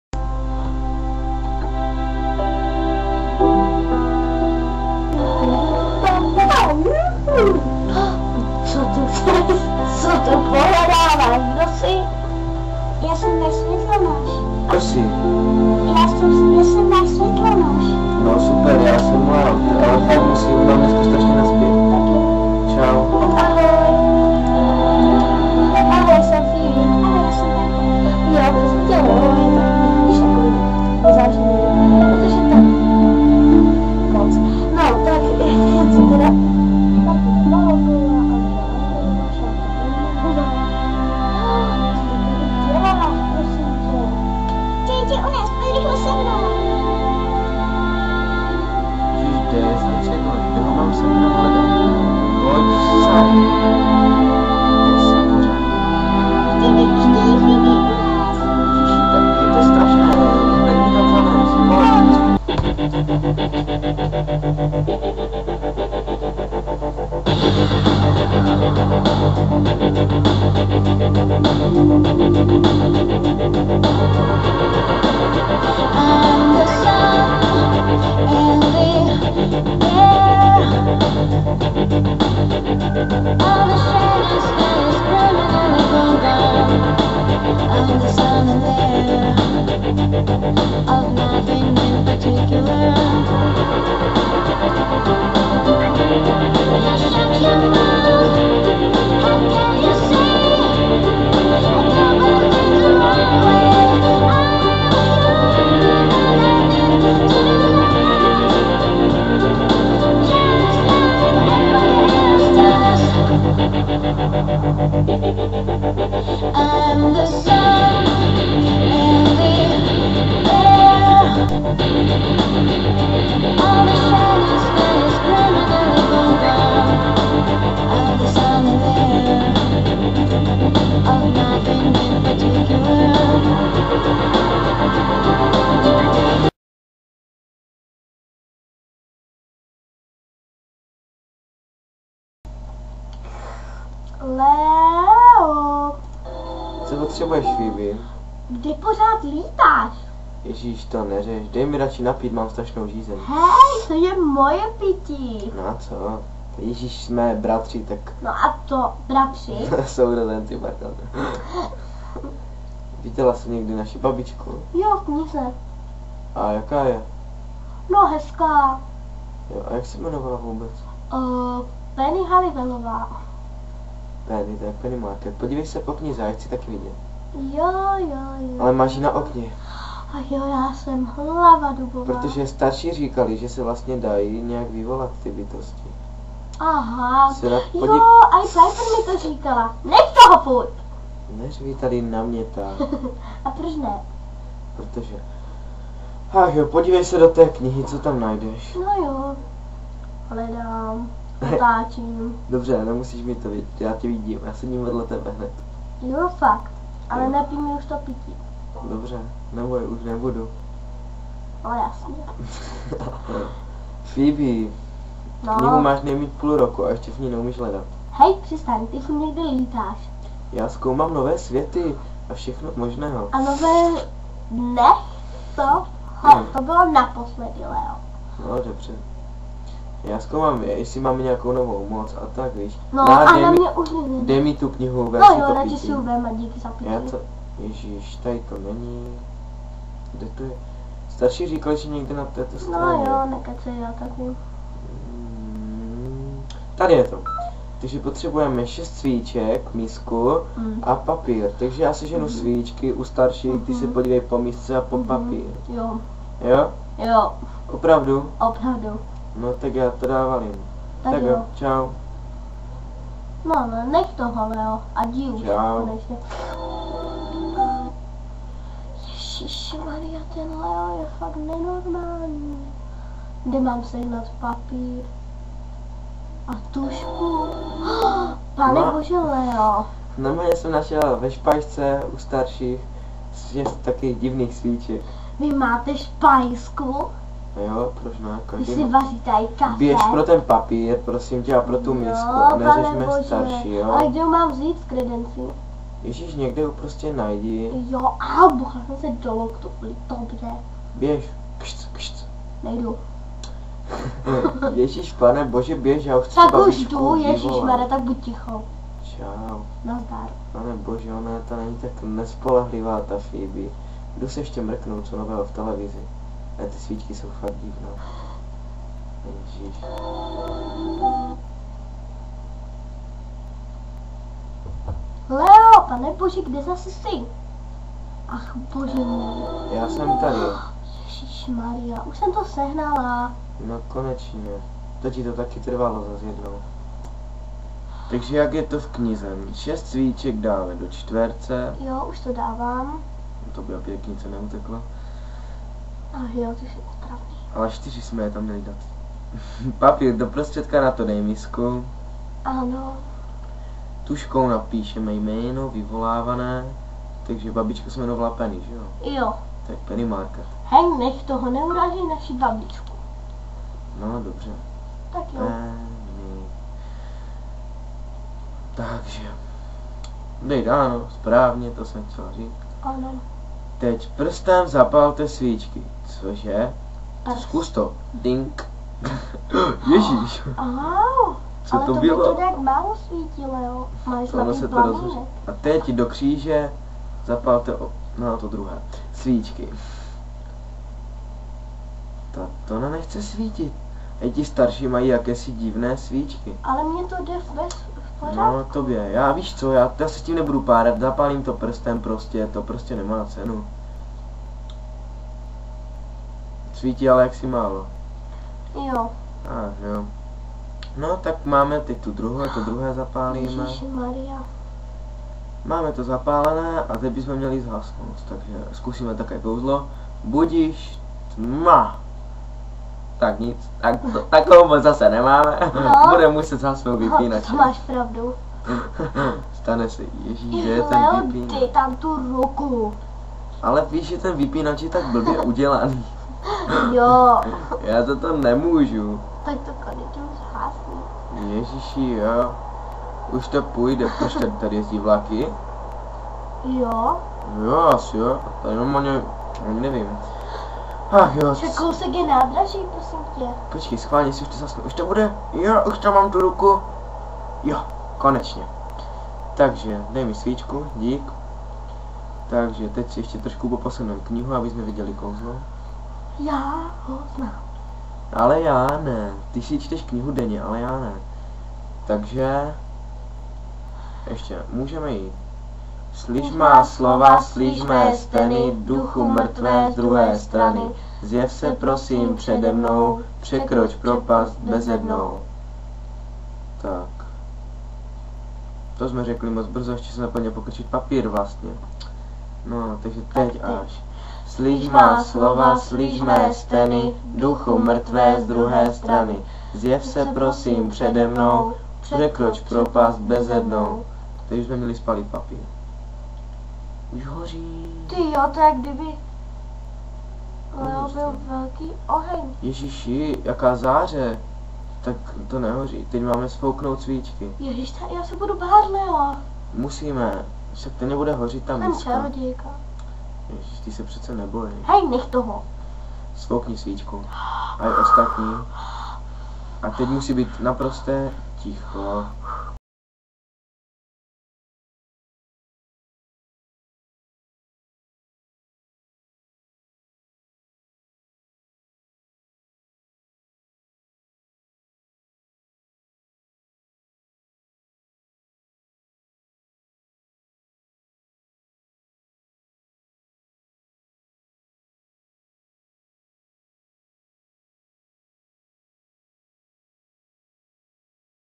3.dil..Babička (1.část)
tentokrat se Phoebe s Leem pokousi vyvolat babičku